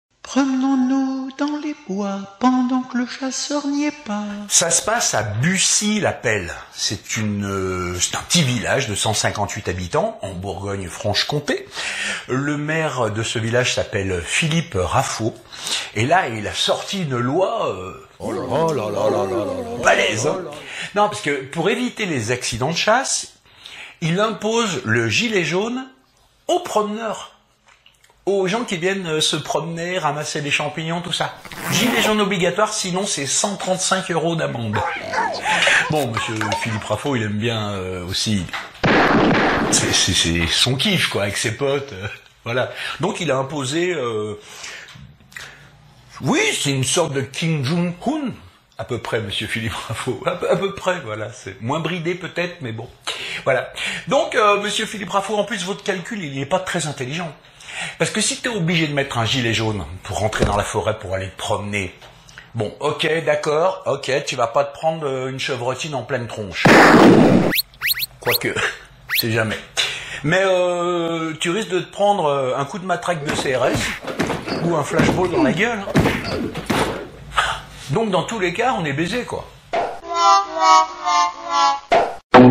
Chasse et promeneurs en forêt
Certains Maire de villages français marchent sur la tête. La preuve !!

Merci d'avance  ;-)